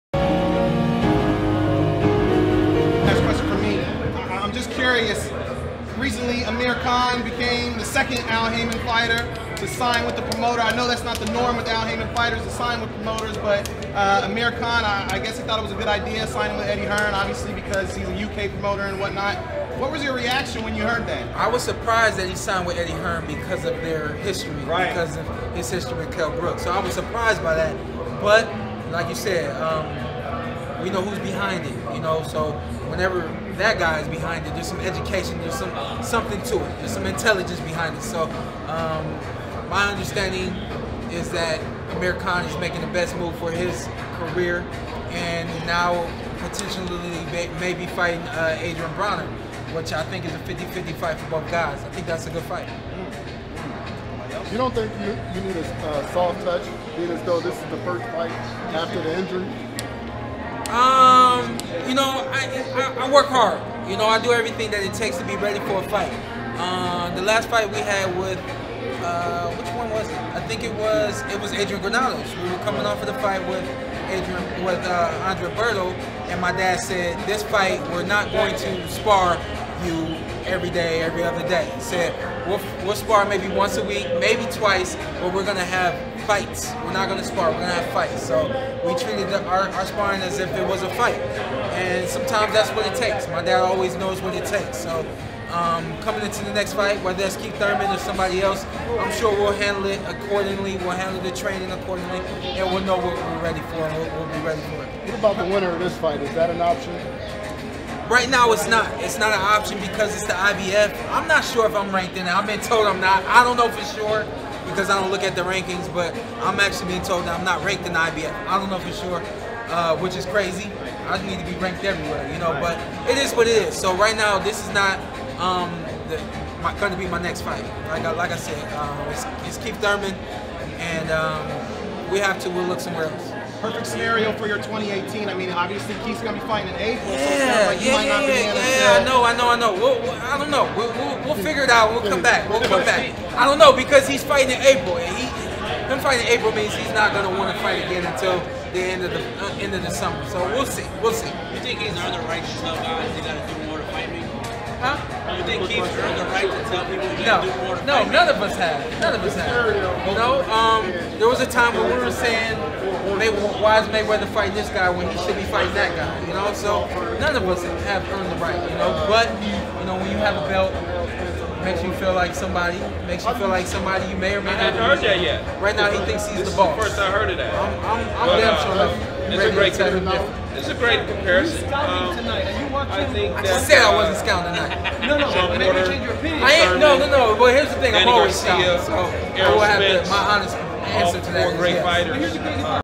PORTER SAYS "NOT RANKED BY IBF" CANT FIGHT SPENCE? TALKS KHAN HEARN SIGNING! KHAN BRONER IN WORKS?
- Shawn Porter On Amir Khan Signing With Eddie Hearn - Khan vs Broner in the Works? - Thurman vs Vargas Preview -

👈 for latest boxing news, rankings, schedules, highlights, reviews, & results **